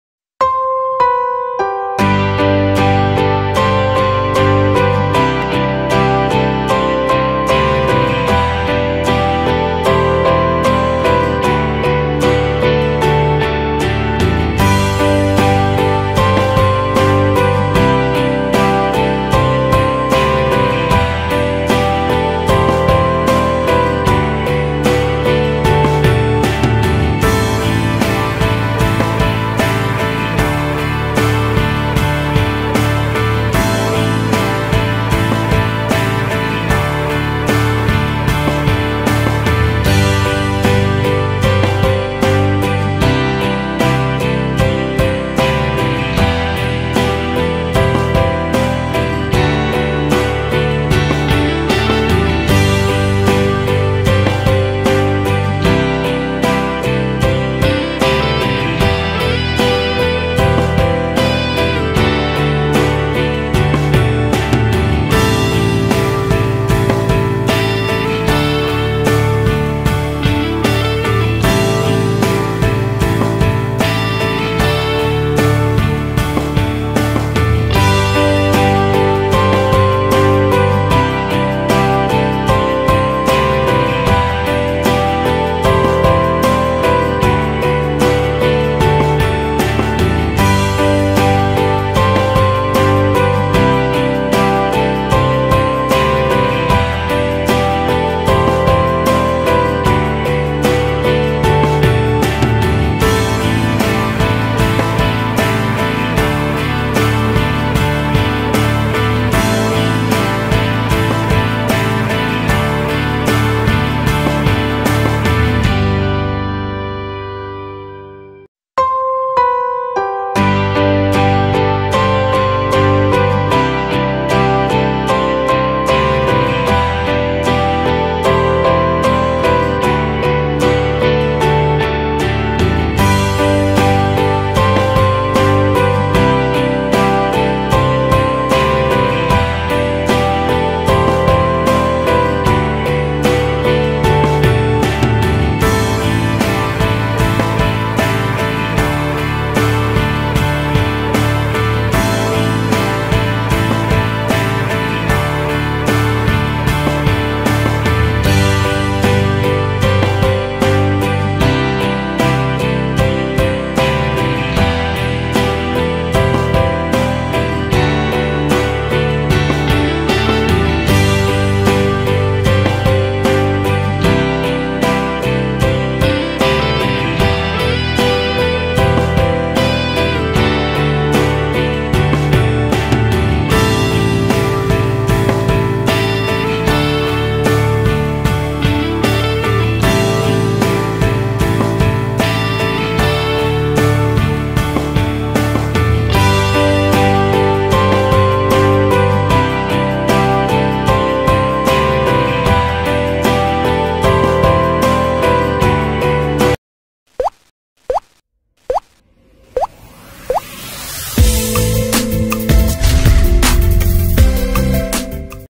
동물접기/강아지/고양이/여우/easy origami/animal origami
유아도 할수있는 아주 쉬운 동물접기에요

강아지, 고양이, 여우 모두 쉽고 귀여우니 따라접어보세요^^